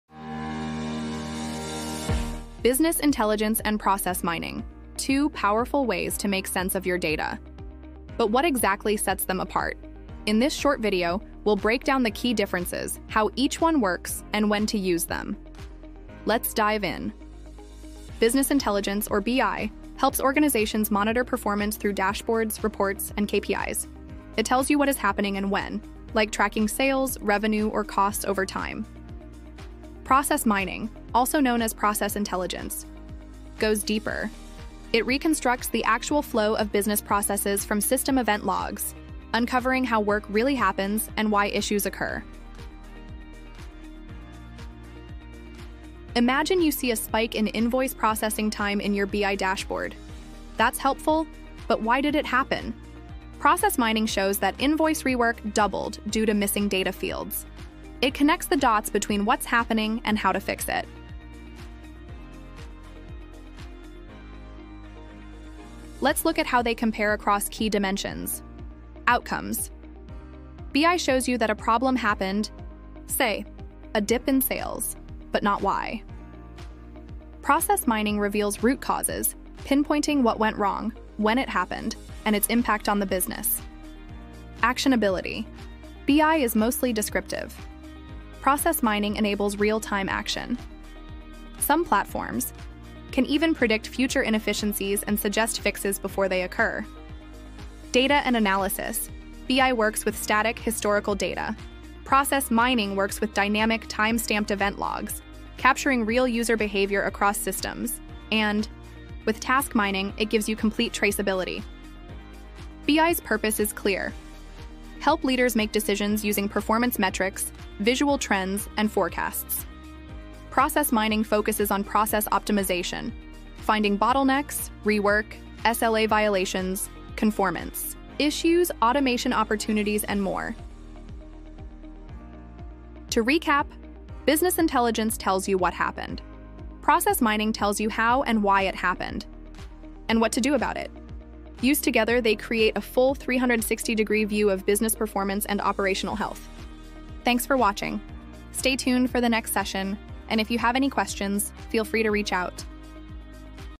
Process Mining vs Business Intelligence: Key Differences Explained
📊 Tired of long theory, dummy examples and no results in process mining?
Learn the pragmatic approach with real use cases and realistic event logs.
In this video, we break down the key differences between Business Intelligence (BI) and Process Mining, helping you understand how each approach supports business analysis and decision-making.

While BI tools answer “What is happening?” using dashboards and aggregated data, Process Mining goes a step further to uncover “Why it is happening” by analyzing real process flows, bottlenecks, and inefficiencies.

🔍 What you'll learn:

The role of BI in reporting and monitoring KPIs

How Process Mining reveals hidden process issues and root causes

Real-world examples comparing both approaches

Why Process Mining is essential for continuous improvement and automation

TIMESTAMPS

0:00 – Introduction
0:17 – What is Business Intelligence (BI) Used For?
0:28 – What is the Purpose of Process Mining?
0:43 – Real-World Example: BI vs. Process Mining in Action
1:05 – Key Differences: Outcomes, Actionability, Data, and Use Cases
2:11 – Summary & Final Takeaways